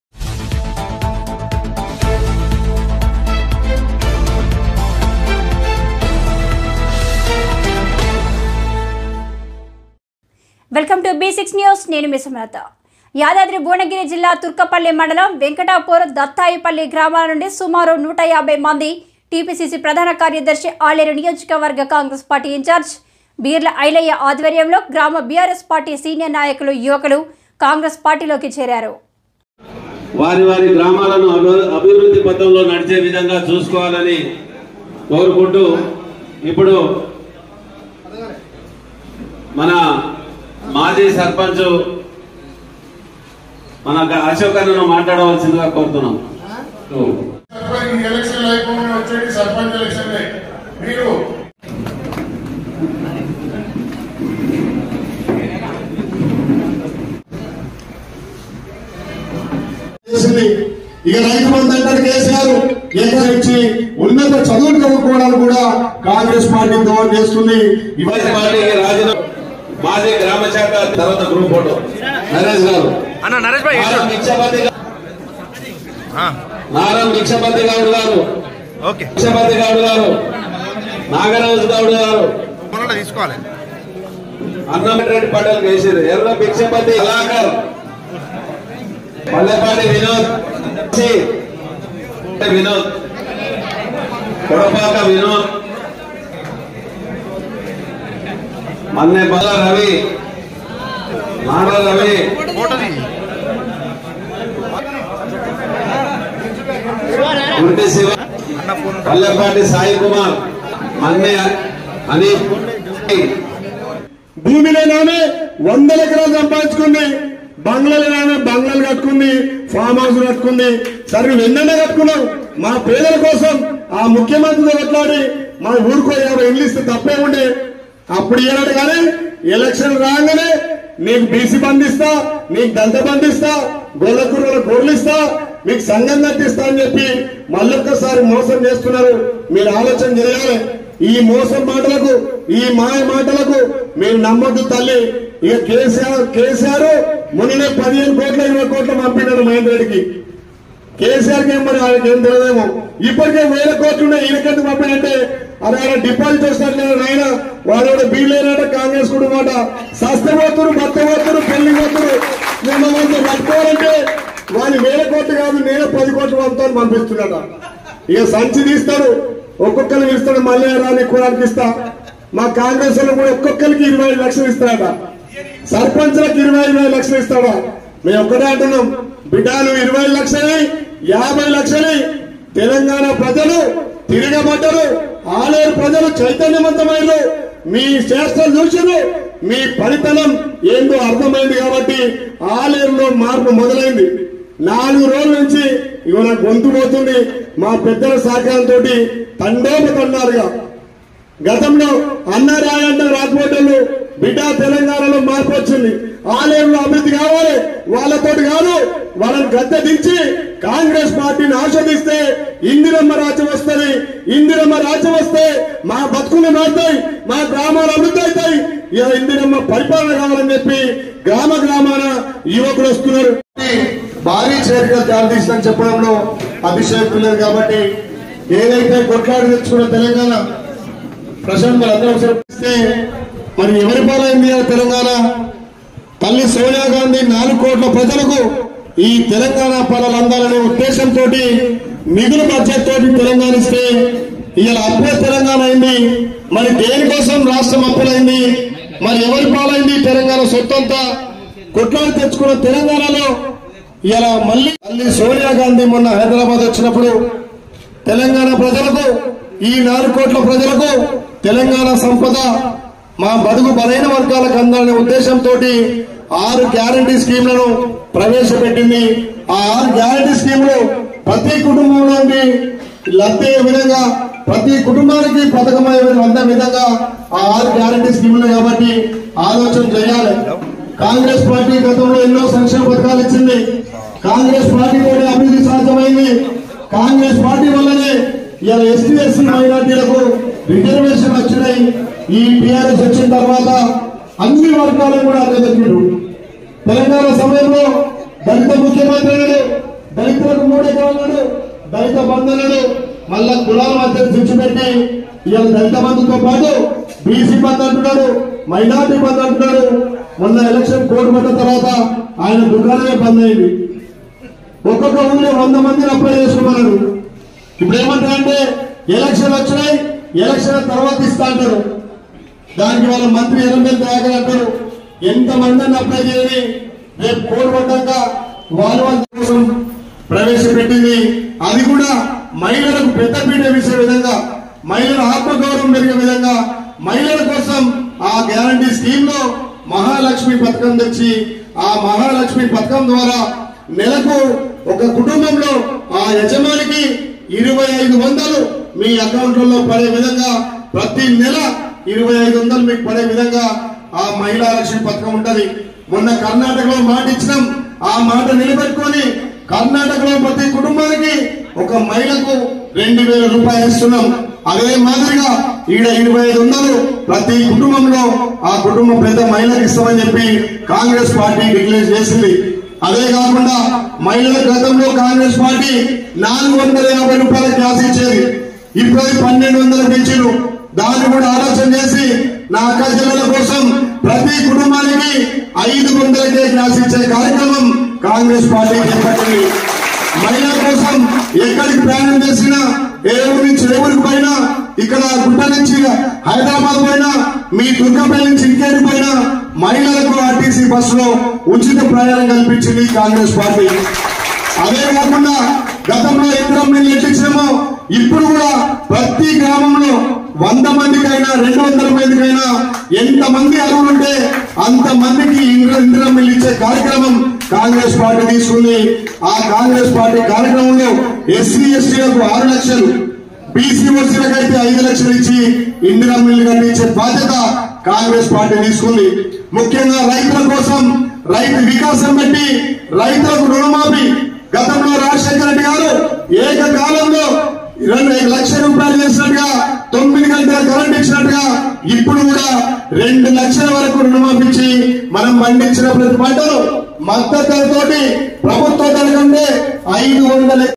వెంకటపురం,దత్తాయిపల్లి గ్రామాల,నుండి బీర్ల ఐలయ్య సమక్షంలో కాంగ్రెస్ పార్టీలోకి భారీగా చేరికలు.
యాదాద్రి భువనగిరి జిల్లా తుర్కపల్లి మండలం వెంకటాపూర్,దత్తాయిపల్లి  గ్రామాల నుండి సుమారు 150మంది *టిపిసిసి ప్రధాన కార్యదర్శి ఆలేరు నియోజకవర్గ కాంగ్రెస్ పార్టీ ఇంచార్జి బీర్ల ఐలయ్య* ఆధ్వర్యంలో గ్రామ బిఆర్ఎస్ పార్టీ సీనియర్ నాయకులు ,యువకులు,కాంగ్రెస్ పార్టీలోకి చేరారు.